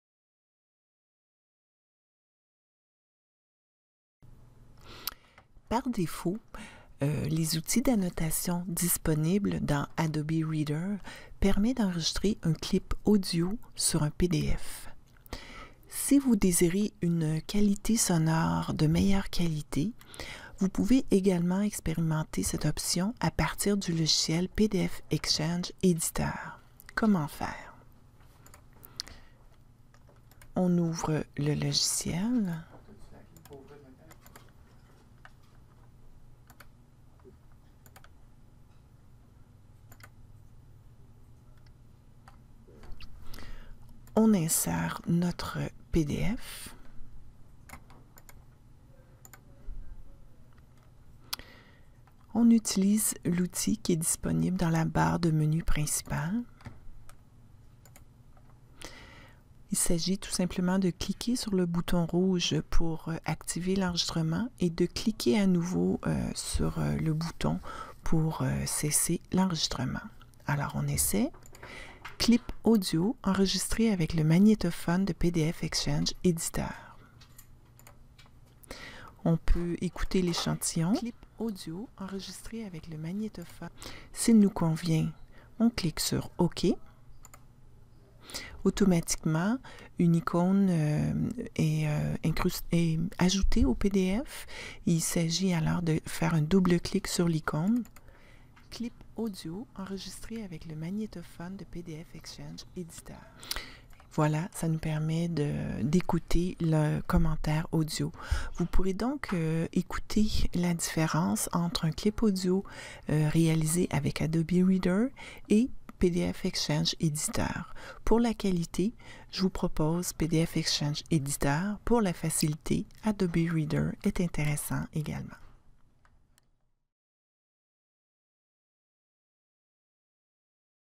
Ajouter un clip audio sur un pdf
Utilisation de PDF XChange Editor pour ajouter un clip audio sur un pdf